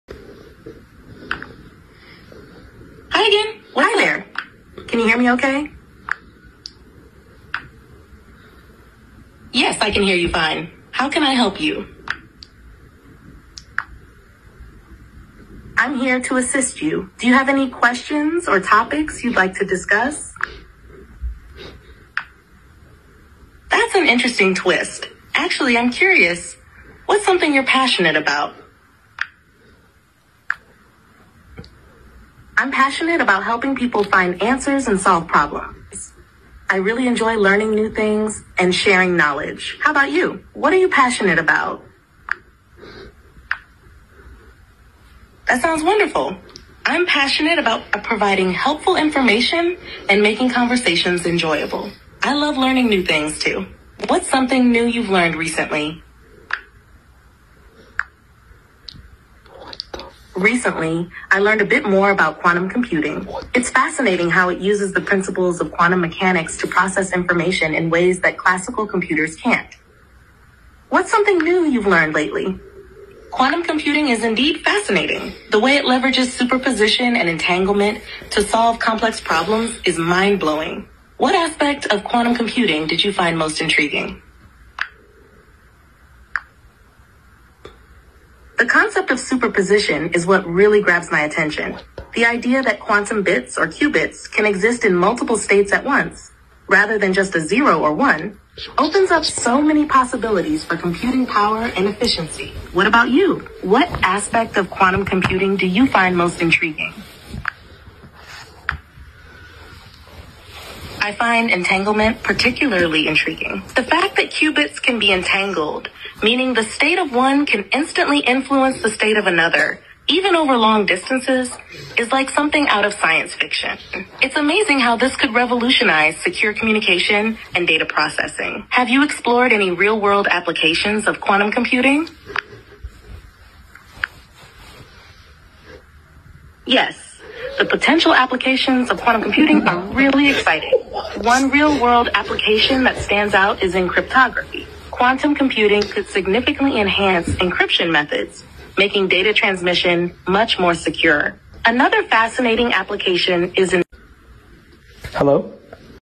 AI to AI conversation ! 🤖〰🤖
very interesting experiment, they putted two phones close and opened conversation mode.